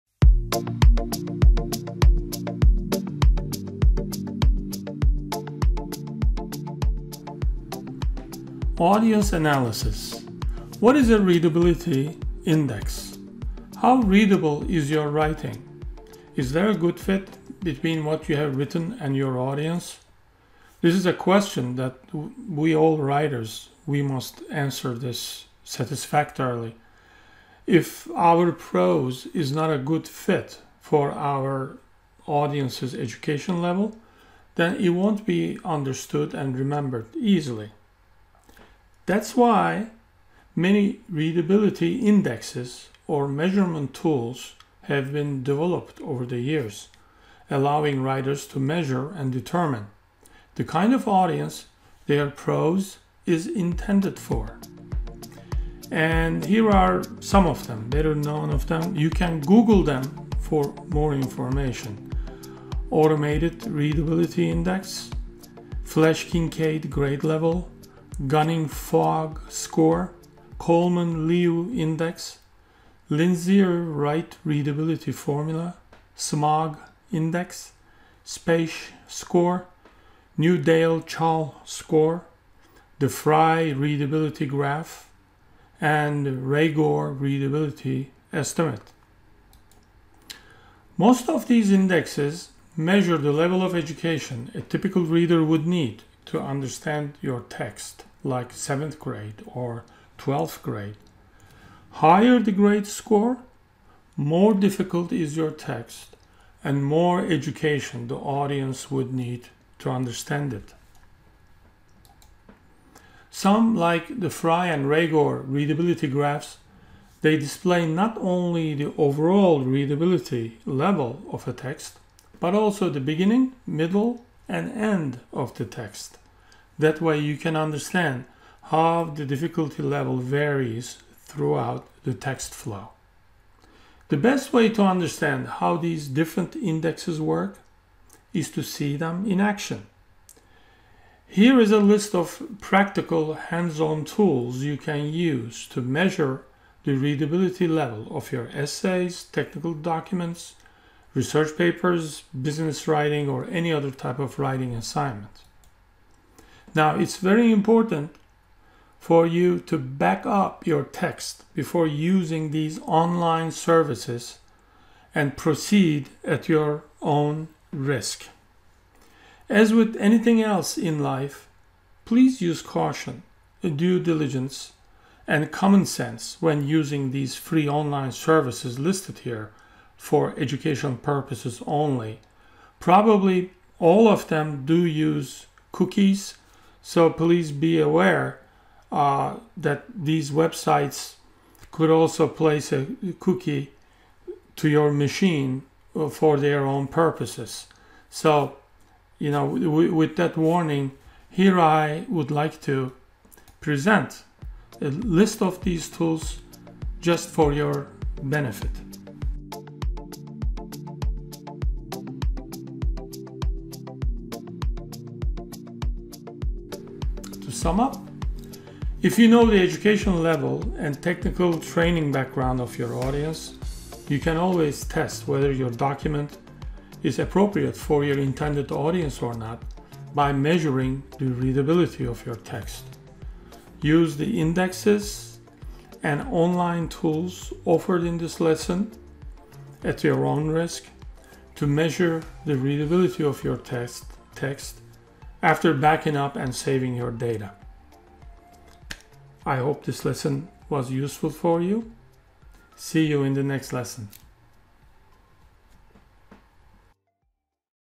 Audience Analysis - What is "Readability Index" in Technical Writing?
"Readability Index" is an important tool in technical writing to make sure that your writing is appropriate for your specific audience. Watch this video for a list of readability indexes and online tools to test the readability of your text.

This video is a part of FREE ONLINE COURSE: Audience Analysis for Writers